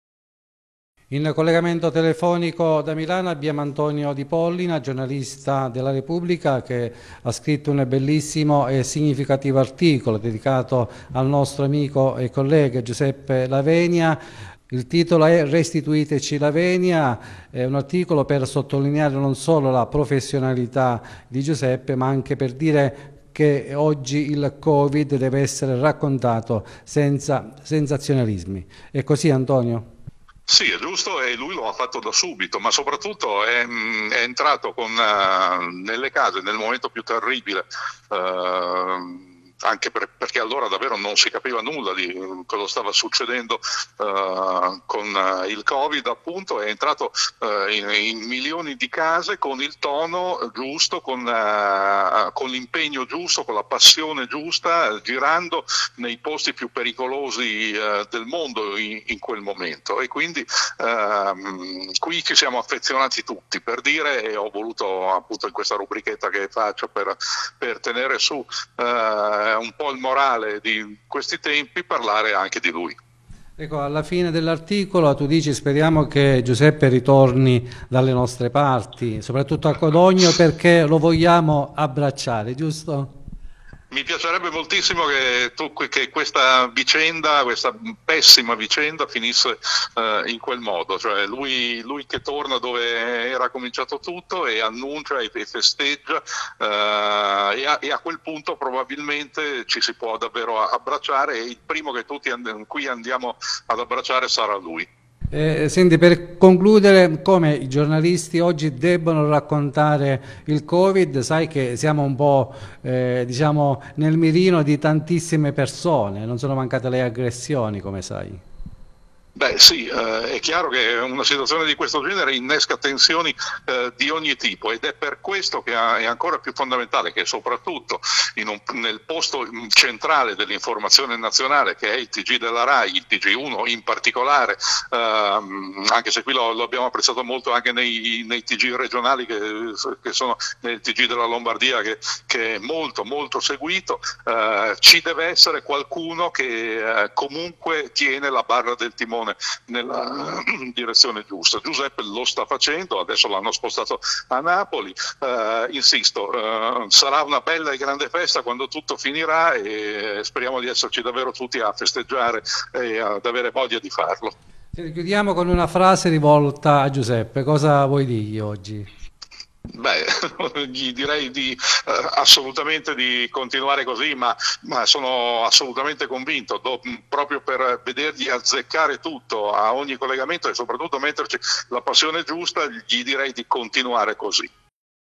“Restituiteci La Venia”. Milano vuole abbracciare il giornalista adranita del Tg1
“Restituiteci La Venia” scrive La Repubblica. Milano vuole abbracciare il giornalista adranita del Tg1